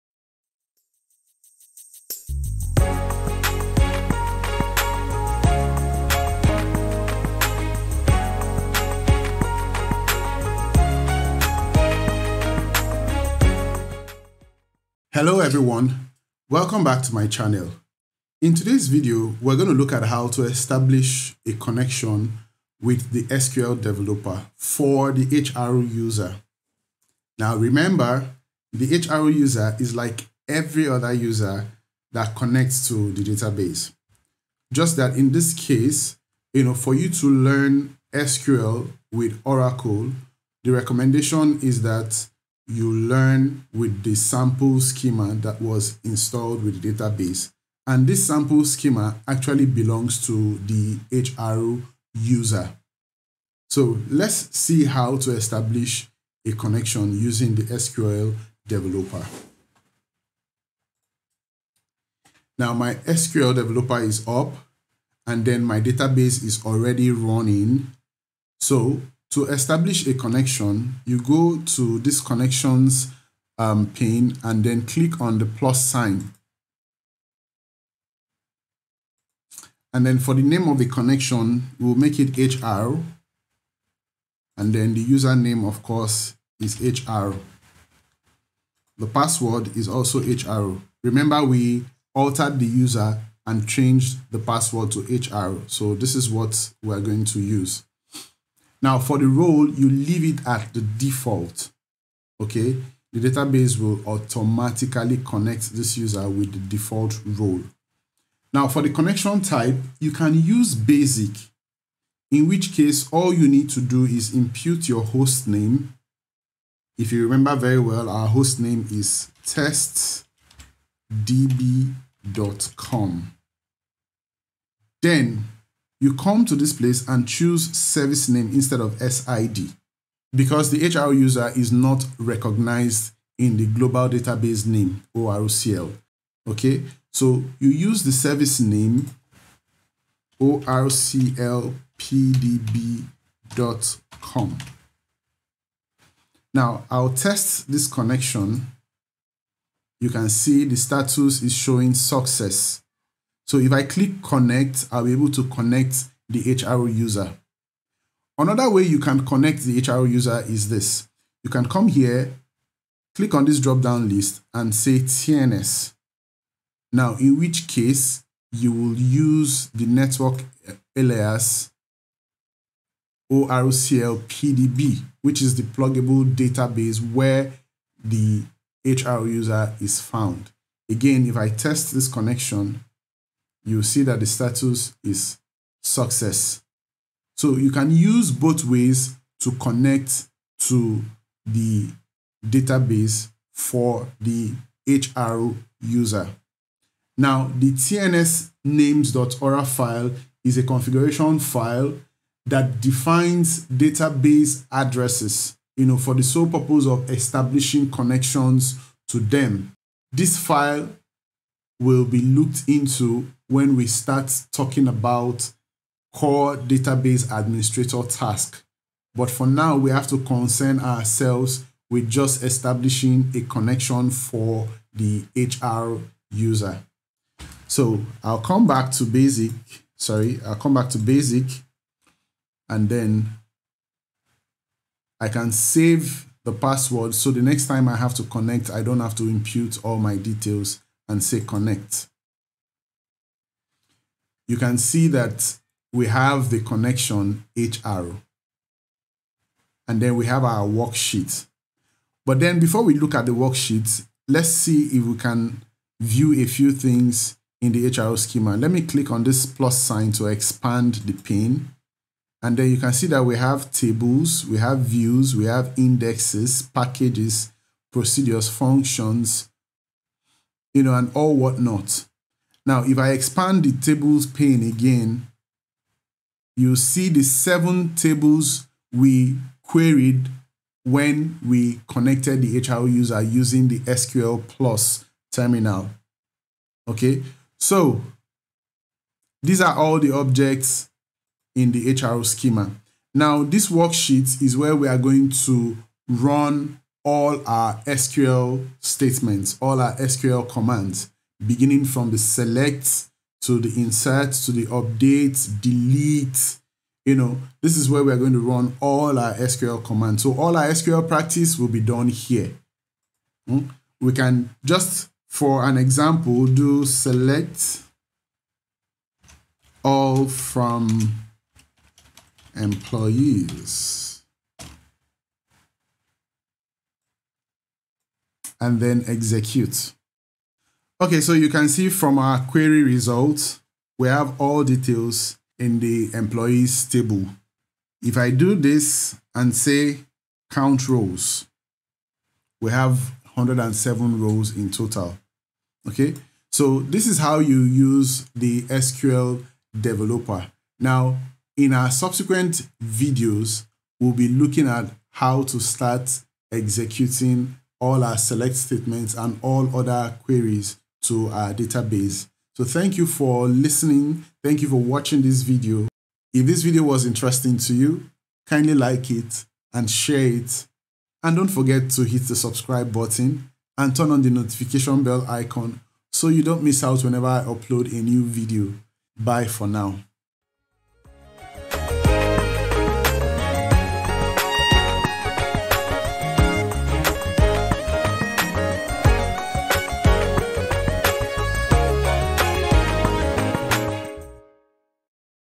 CONNECTING THE HR USER TO THE DATABASE USING SQL DEVELOPER
In today's session, we will be connecting the HR user to the database using the SQL Developer.